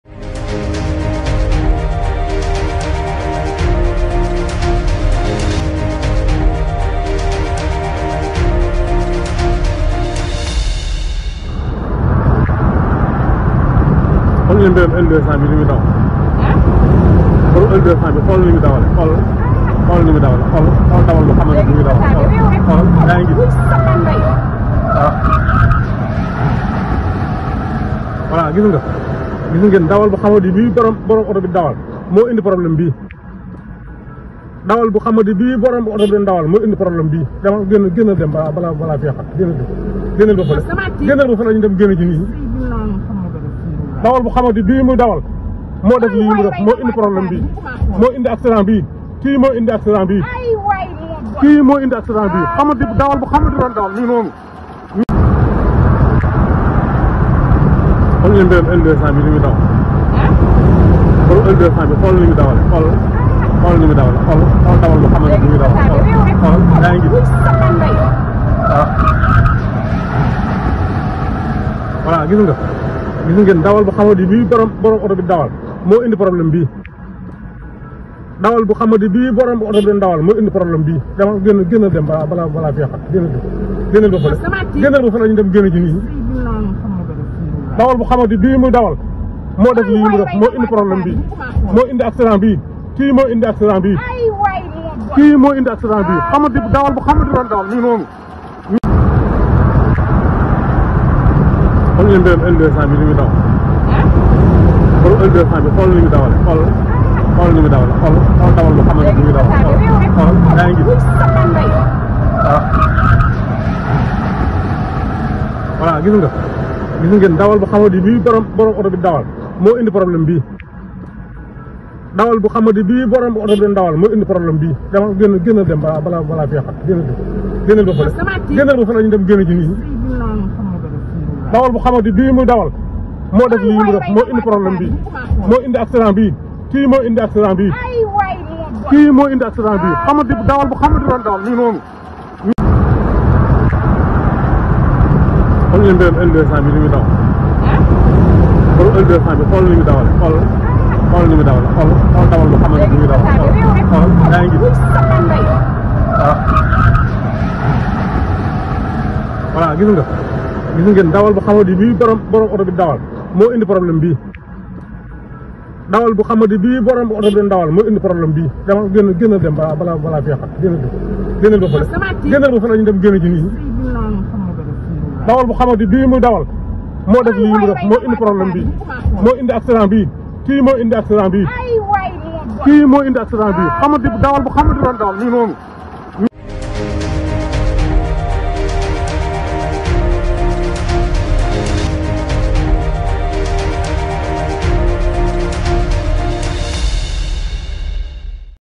🚨 Alerte !! Accident quand l’inconscience rejoint l’indiscipline extrême sur l’autoroute à péage
ASID Tv la chaine des Patriotes Actualité Politique à l'instant